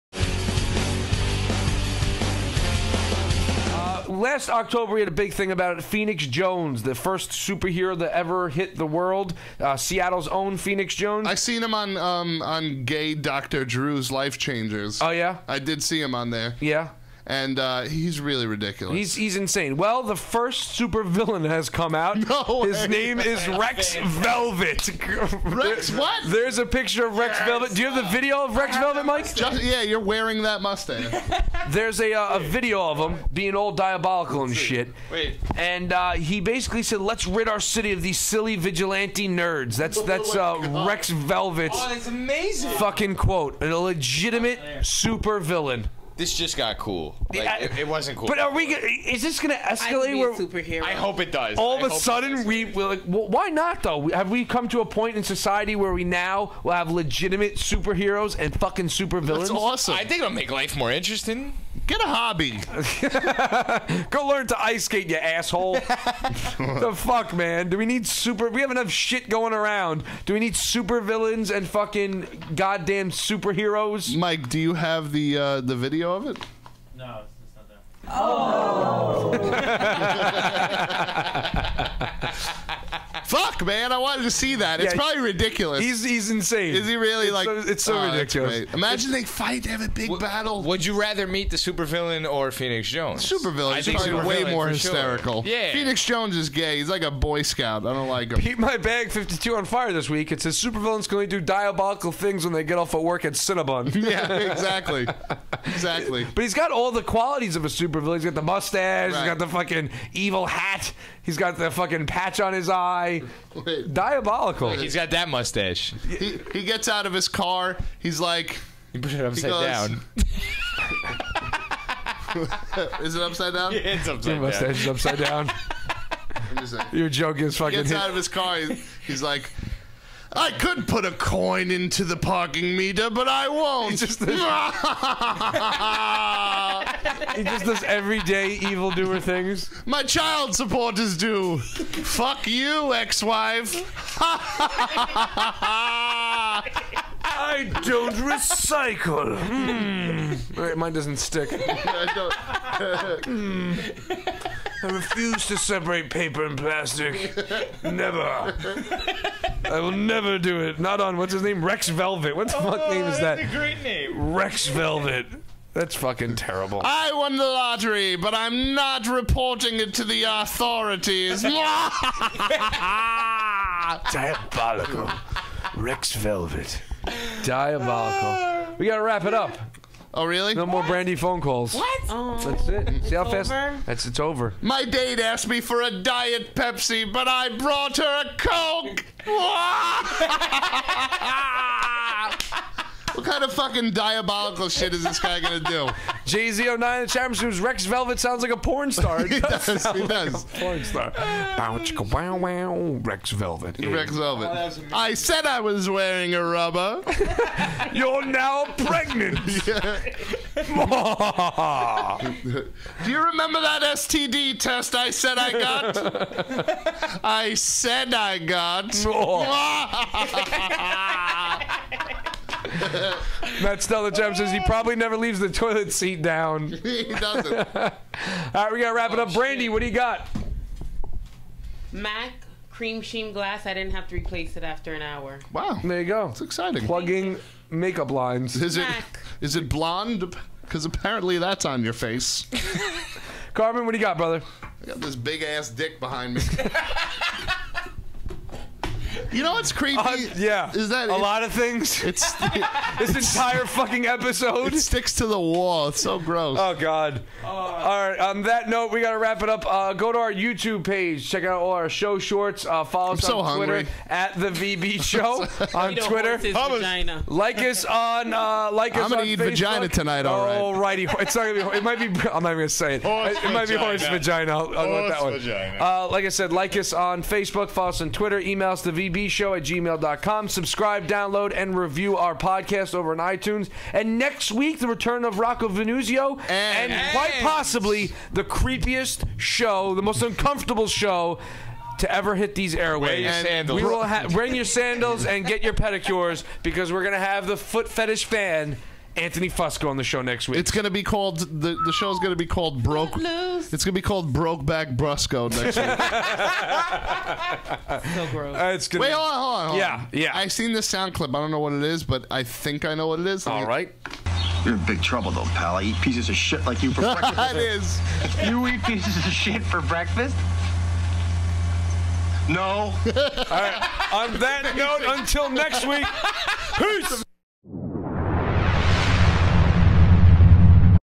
The Vinny Brusco Show: Rex Velvet, Everyday Supervillain
The Boys and Girl discuss the world's first supervillain, besides Kris Jenner's Womb, Rex Velvet. Check us out live every Thursday at 10:15pm est on Zenlive.tv